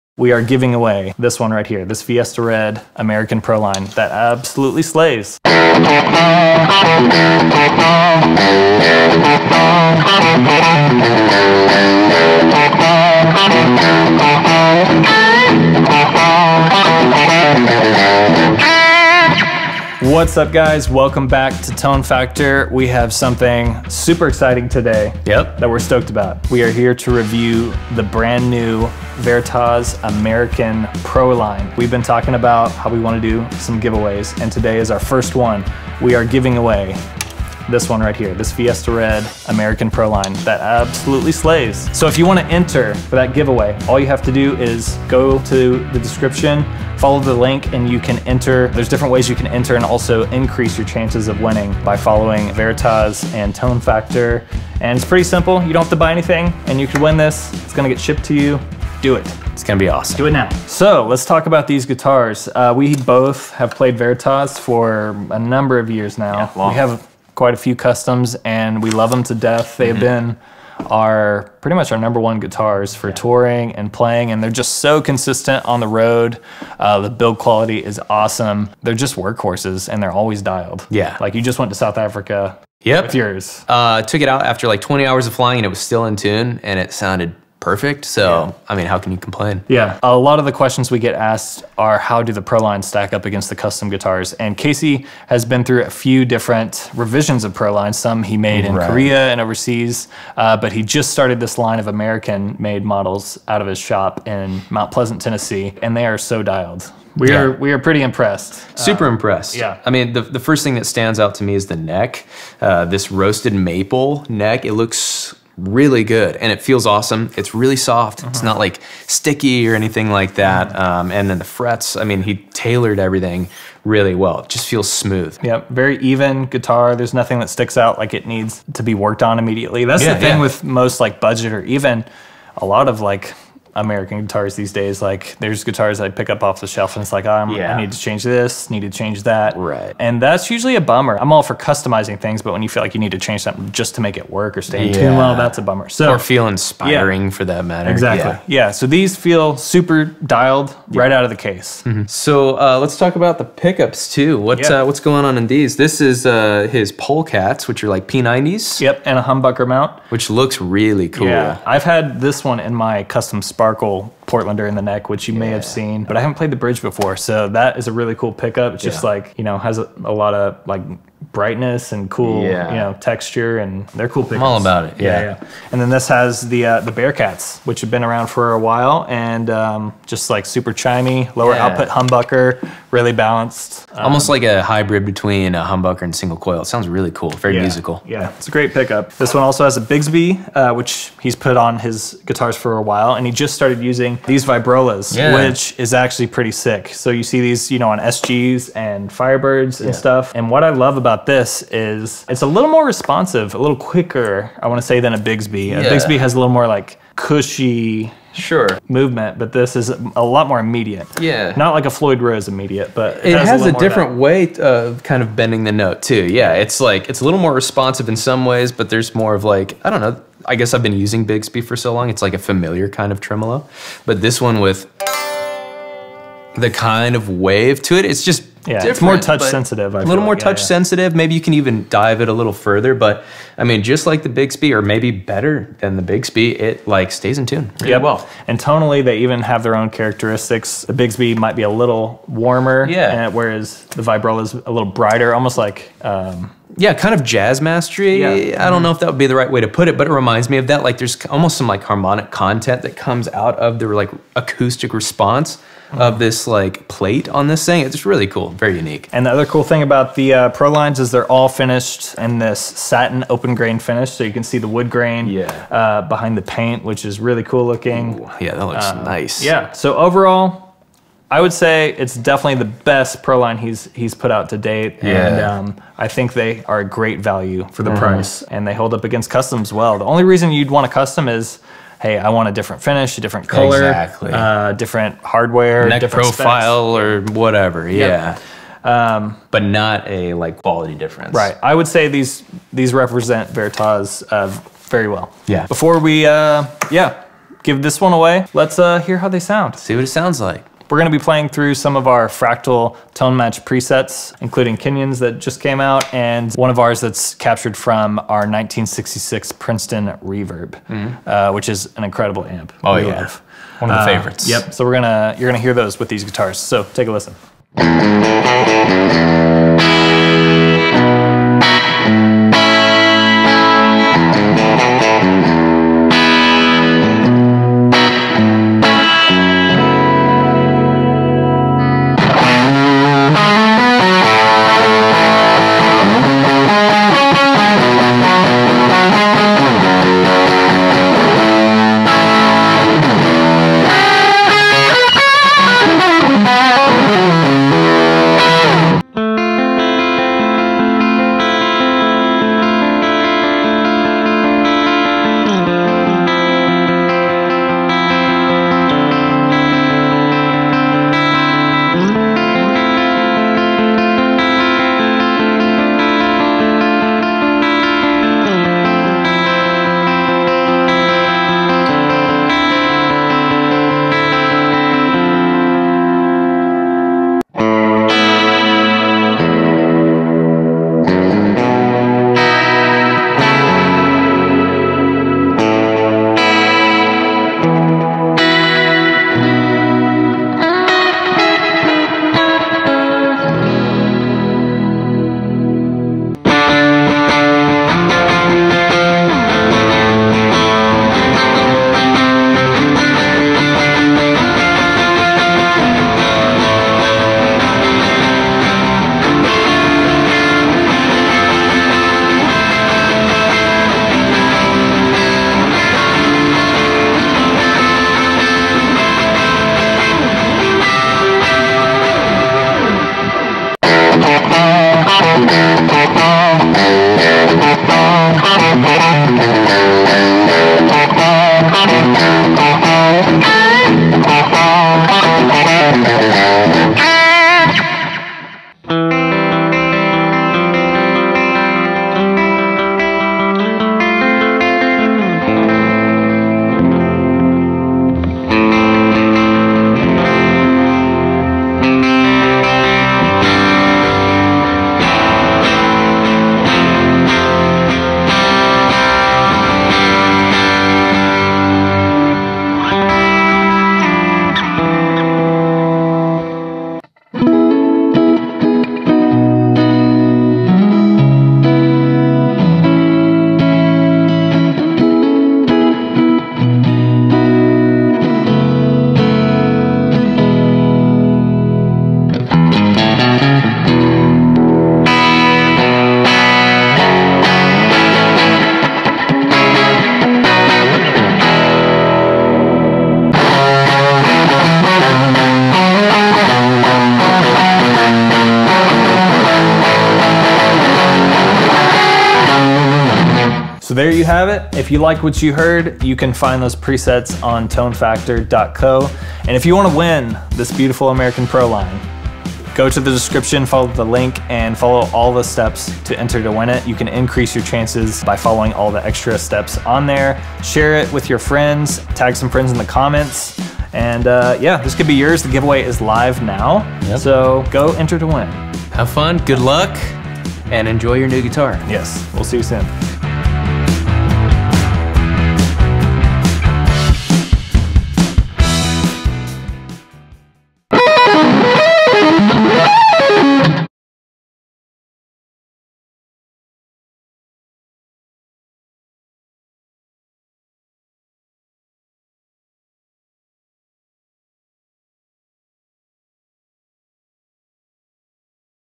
$2500 GUITAR GIVEAWAY! Veritas Portlander American Proline - Giveaway & Review
ENDS MAY 31st, 2023!

Join our Tone Factor Facebook groups & connect with other users:
Main Group